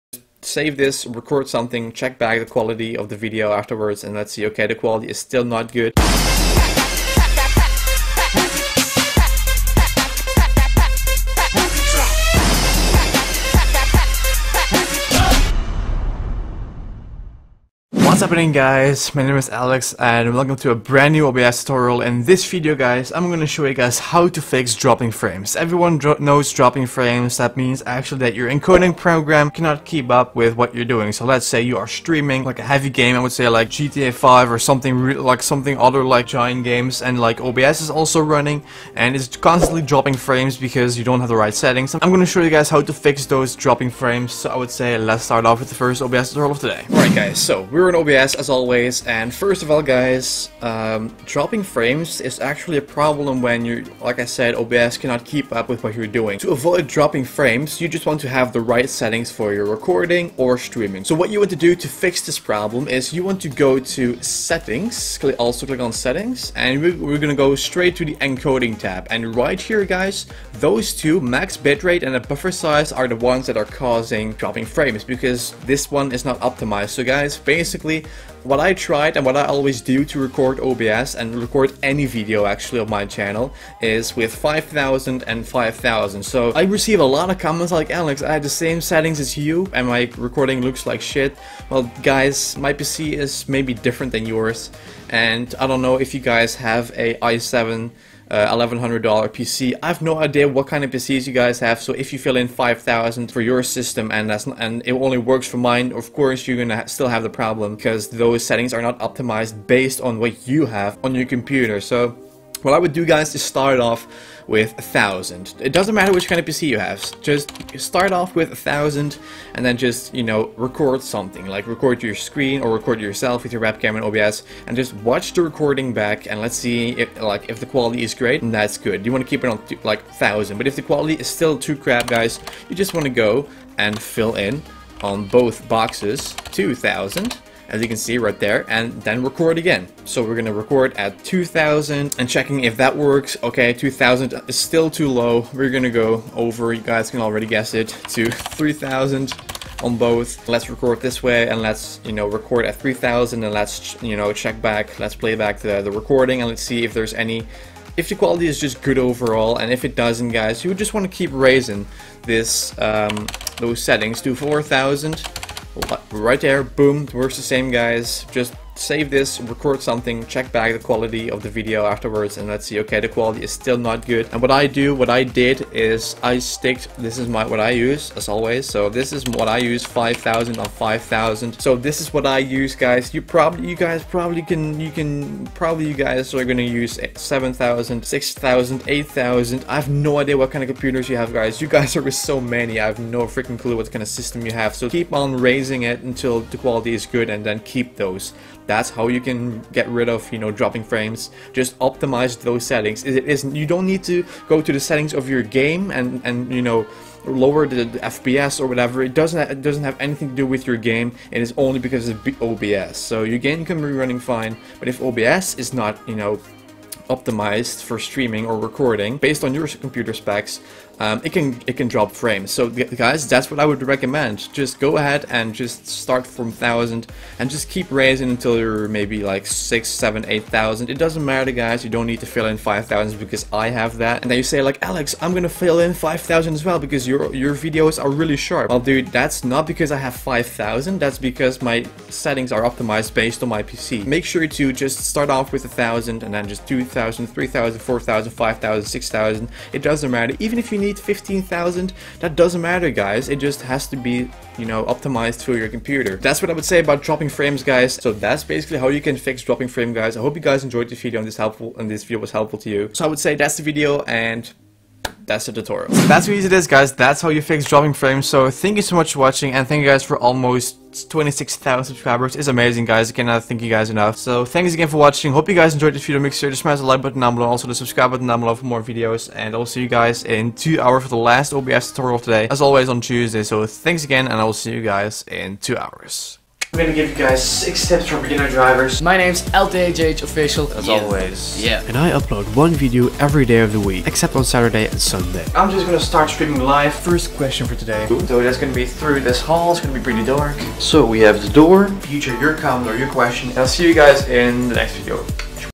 How To Fix Dropping Frames In Open Broadcaster Software - Tutorial #54
- - In today's tutorial I'll be showing you how to fix dropping frames.
I receive a lot of comments and questions about how to do certain things. More tutorials are on its way!

~ JustAlexHalford